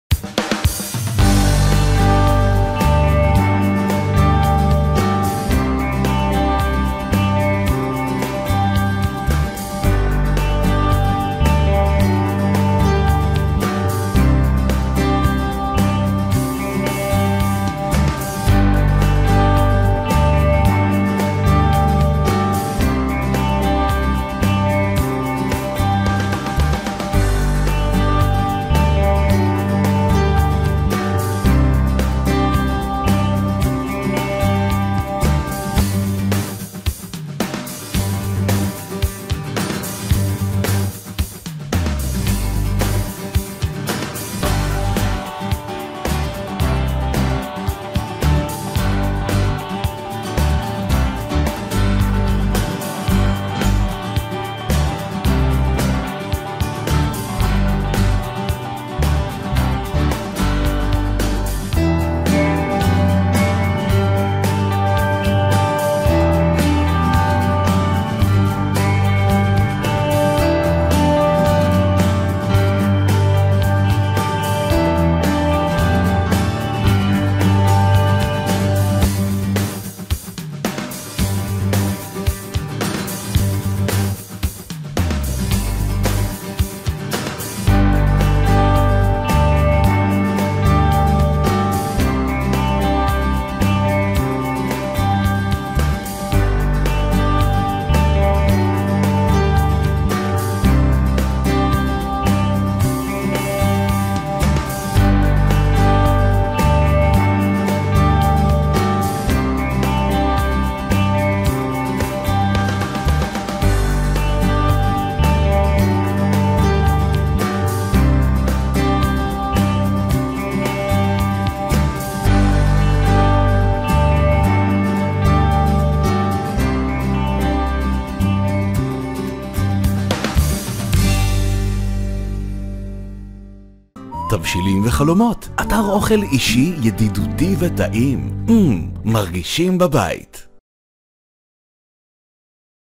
איך מכינים בוריק / בריק / בוריקה במילוי ביצה?
בריק או בוריקה במילוי ביצה? תלוי את מי שואלים. את הטוניסאים או הטריפוליטאים. האמת שאני לא טריפוליטאית ולא טוניסאית אבל אני כן אוהבת קיצורי דרך, קבלו את הגרסה העצלנים להכנת הבריק.